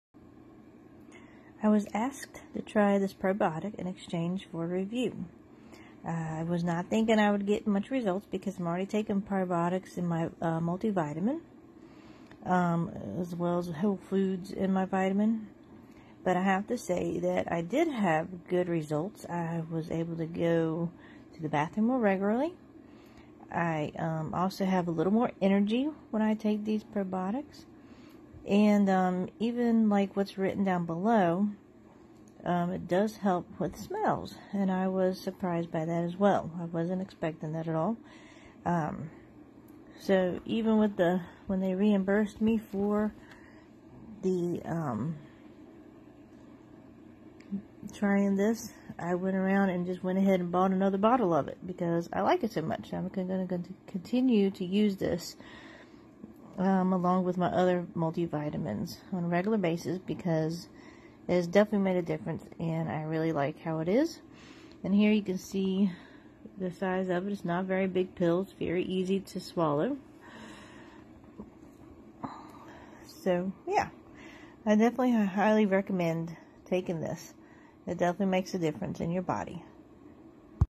So glad I tried this!
I’m gonna be honest here: I only bought these Probiotics because I knew I would be reimbursed for them. Let’s be real, who would ever say no to some free stuff?

I already take probiotics since they’re included in the formula of the multivitamins I take, but this is my first time trying out a supplement that revolves solely around probiotics. I didn’t know what to expect but as you can see from this review, I am FAR from disappointed!!

These really opened my eyes. I thought my multivitamins would be enough to keep me good in all areas of my body, but trying out this probiotic made me realize that my body could be so much better with the right products. Taking these probiotics really let me see the change in myself. My energy levels have been boosted tenfold and my digestive seems to be on the right track, seeing as I’m using the bathroom more regularly these days.

In short: Just. Buy. Them!! Investing in a good quality probiotic could save your body (and your wallet) so much trouble in the long run!